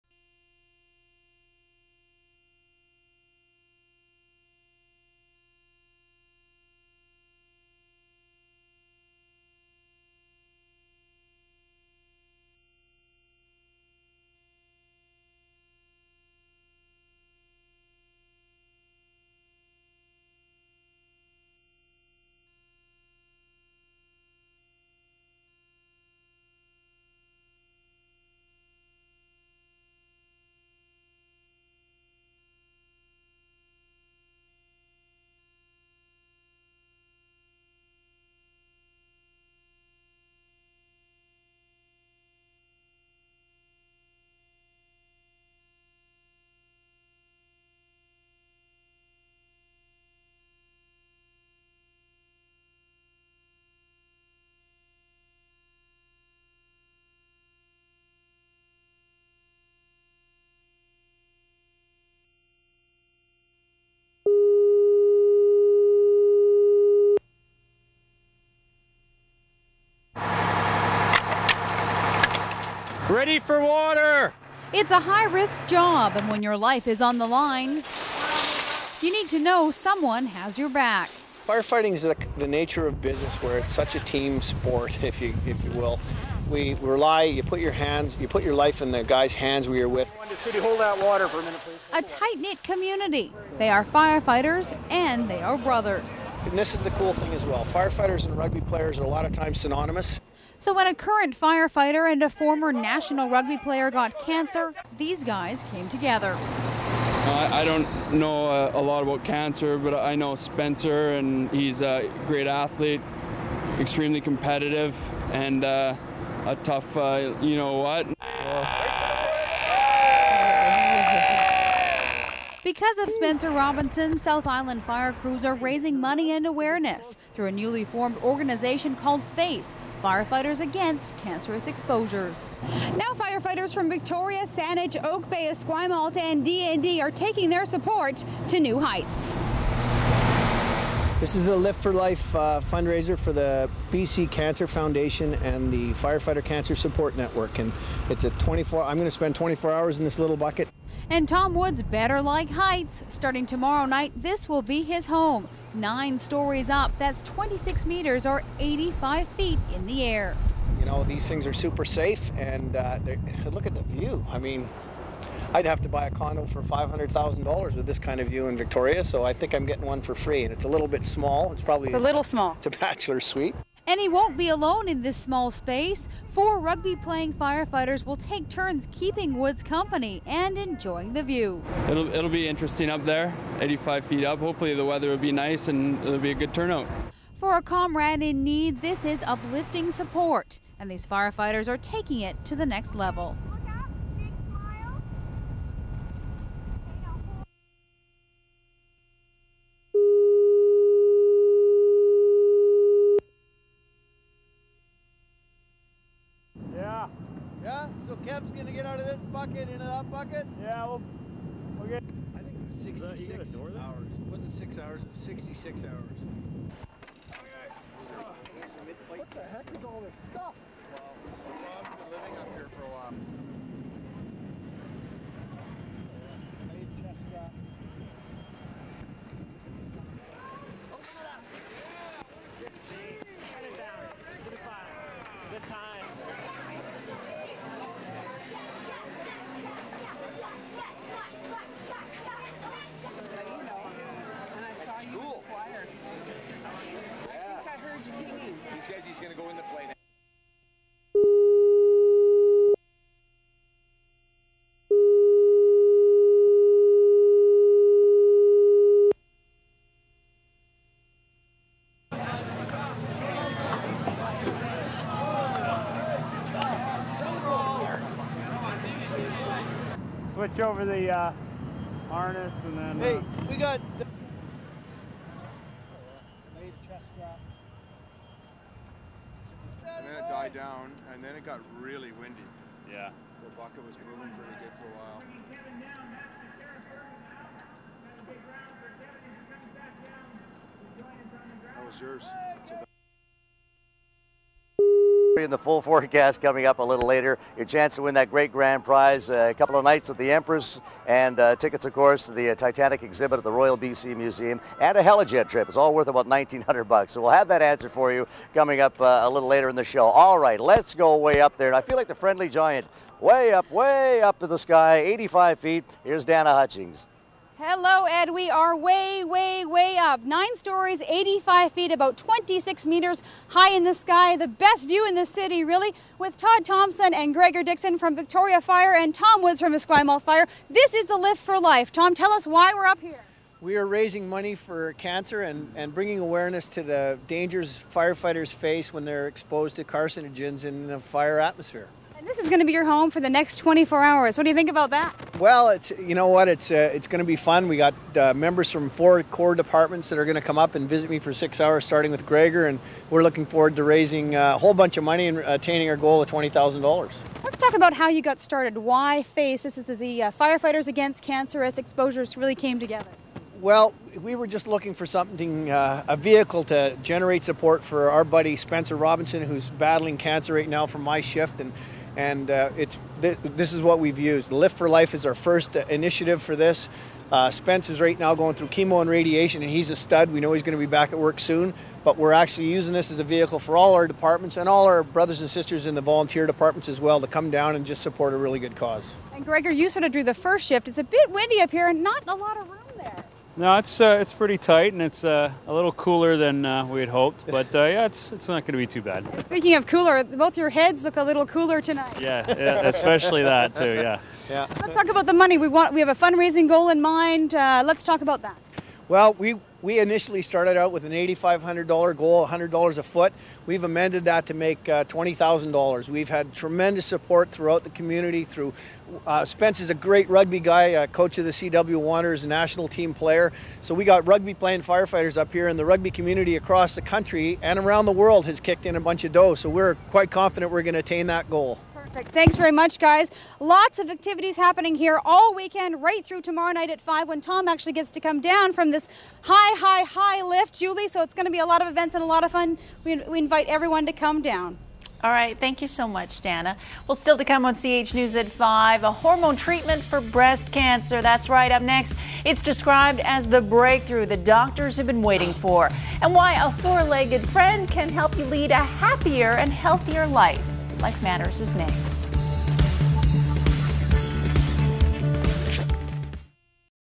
Rugby Players and Firefighters Unite to Battle Cancer
ESQUIMALT - A group that is sometimes on opposite sides on the rugby field has united for Lift for Life, a fundraising event to help Esquimalt firefighter, rugby player and coach Spencer Robinson. Fellow rugby player and firefighter Tom Woods will spend 24 hours 85 feet above Esquimalt Plaza for "Lift for Life", an event to launch F.A.C.E., Firefighters Against Cancerous Exposures.